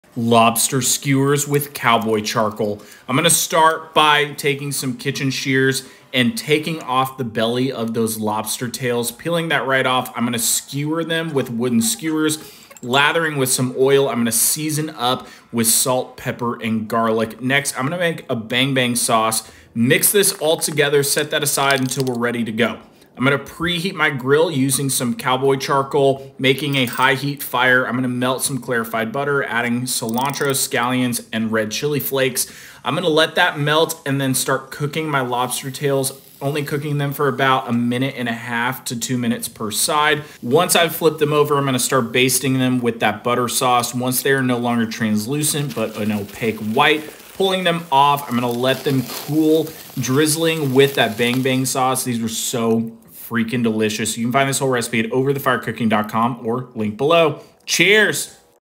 Lobster Skewers with Bang Bang Sauce! 🦞 🌿 🔥
Lobster Skewers with Bang Bang Sauce are out of this world! 🦞 🌿 🔥

Lobster Skewers with Bang Bang Sauce for a fun and delicious dinner. I fired up my grill with some @CowboyCharcoal and cooked these lobster tails over the coals on some skewers. It’s always important to have a consistent heat source when cooking over the fire. It becomes even more important when you’re cooking a meat as delicate as lobster tails. I naturally reached for my Cowboy Charcoal because their charcoal provides exactly that: a clean burning and consistent heat source. I finished these lobster tails with some Bang Bang Sauce and they were so freaking delicious.